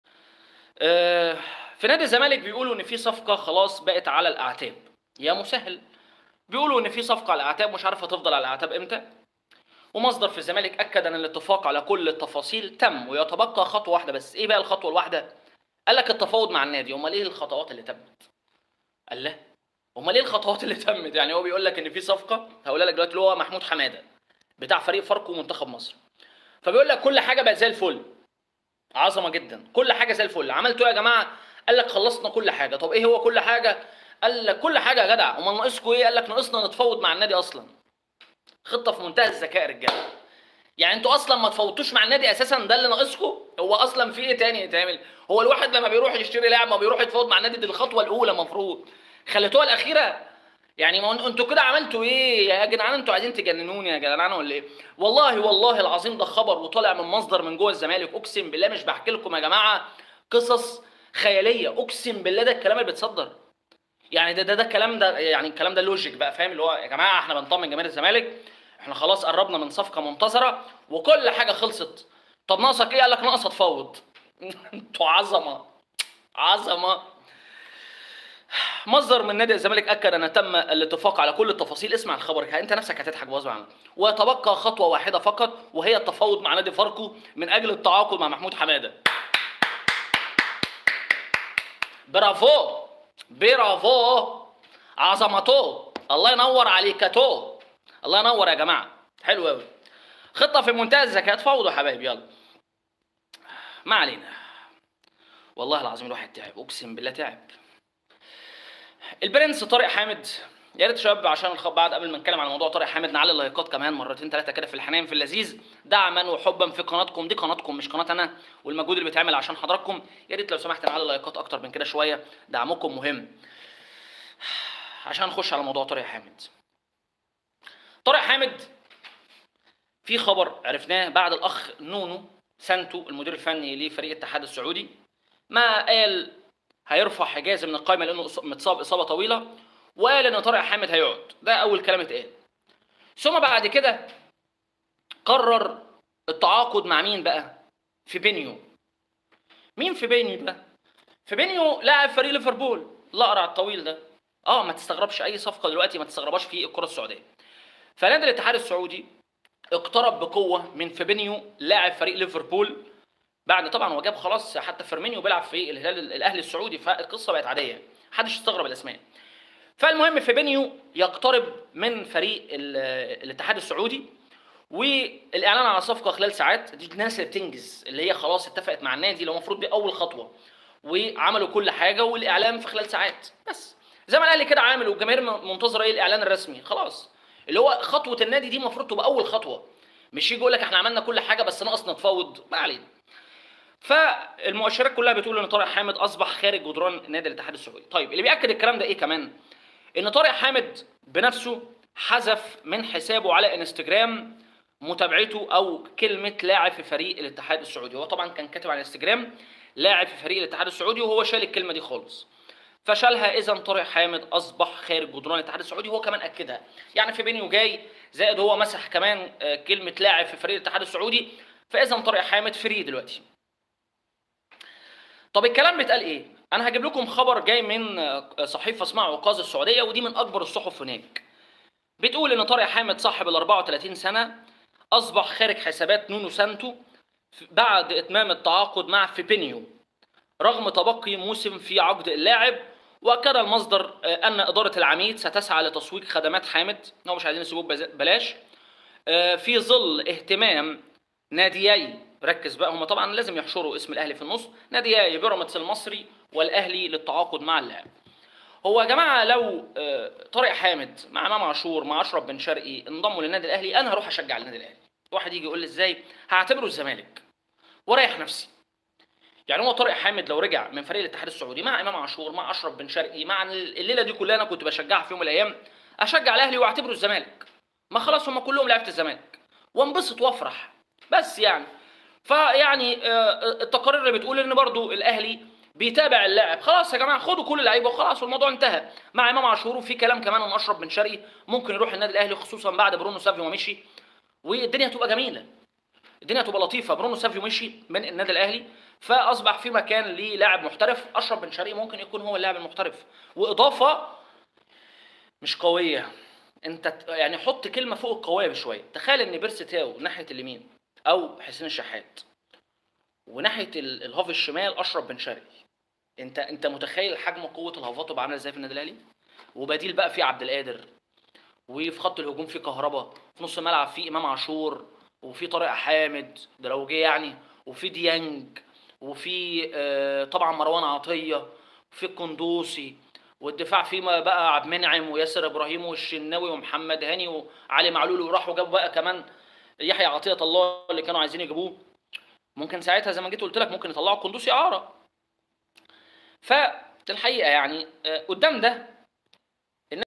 صفقه قويه للزمالك متوقفه على التفاوض والتعاقد | اسلام علوي
شيخ بحه يرد على مرتضى منصور
ما عليك سوى الانتساب إلى هذه القناة للاستفادة من المزايا :